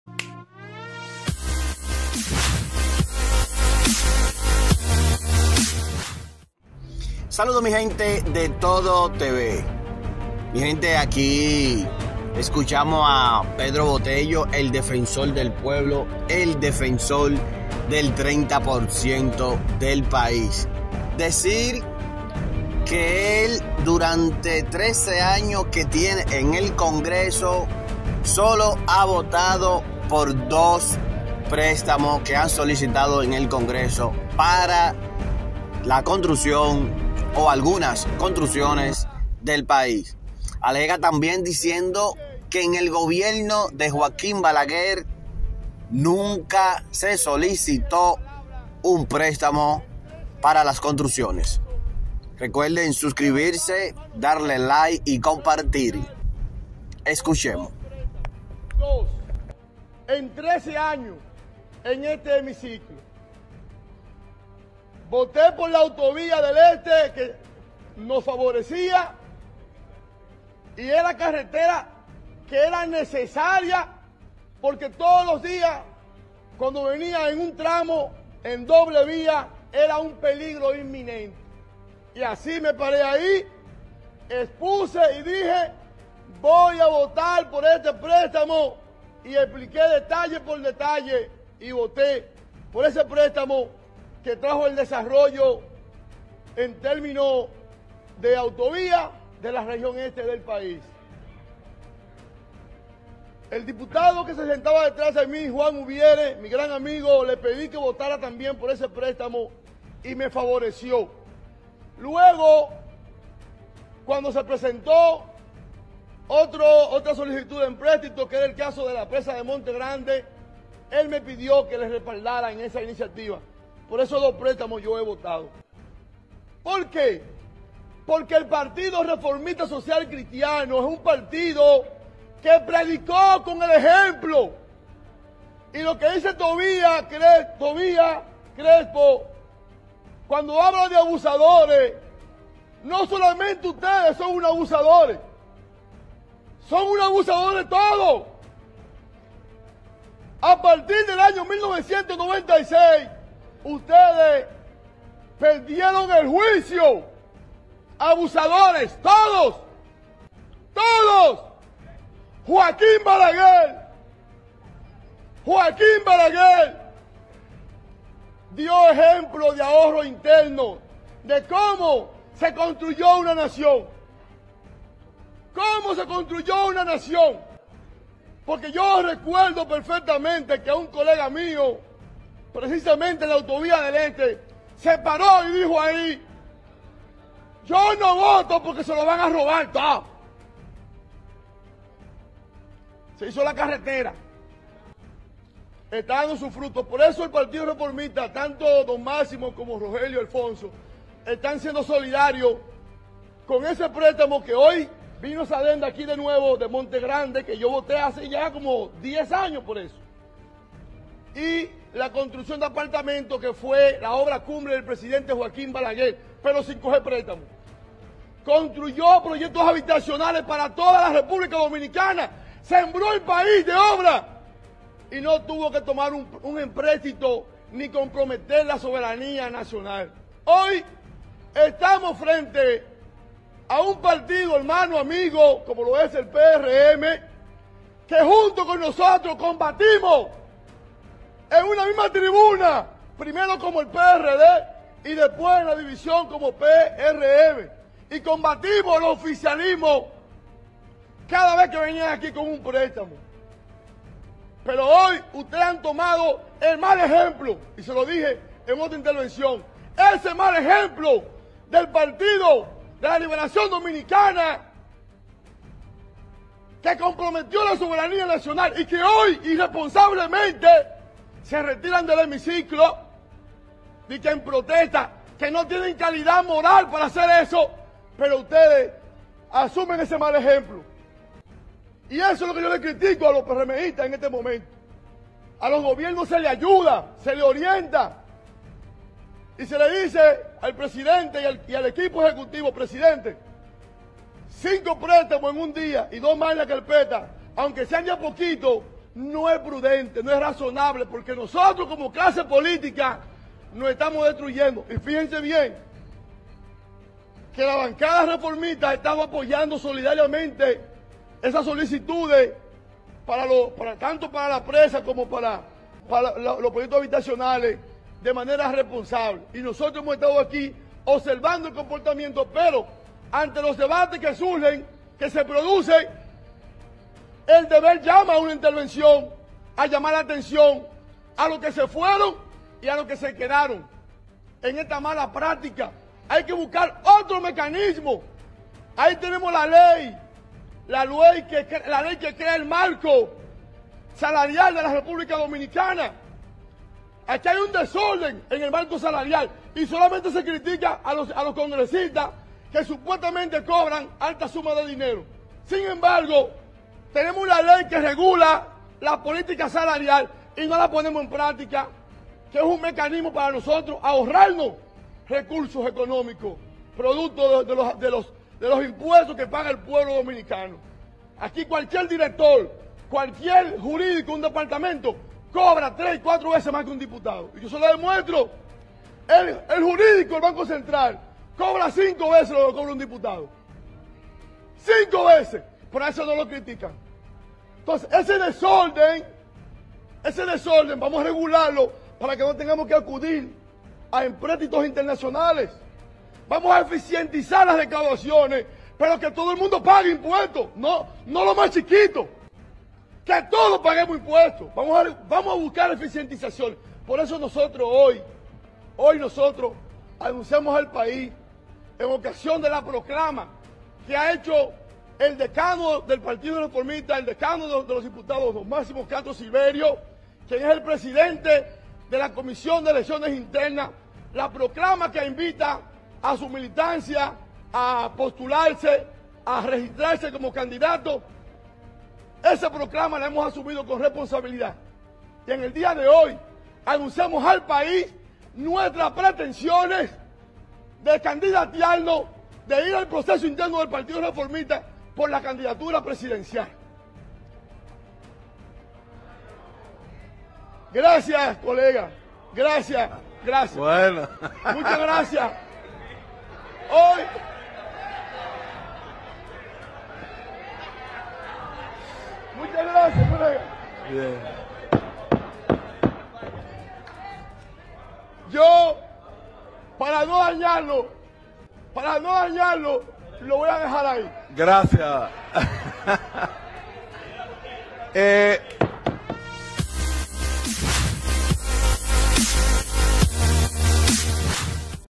💰Pedro Botello Los PRÉSTAMOS UNDEN el PAÍS - Afp
PedroBotello sostiene que durante 13 años que lleva en este Congreso Nacional solo ha votado por dos préstamos. Dice que el PRSC mantiene su postura de no votar a favor de ningún préstamo; expresa que el Dr. Joaquín Balaguer construyó muchas obras sin tomar ningún préstamo.

Cuestiona el mal ejemplo que ha copiado el PRM del PLD, porque no ha sido prudente tomar tantos préstamos en un mismo día. Informa al pleno y al pueblo dominicano que asumirá la candidatura presidencial de su partido.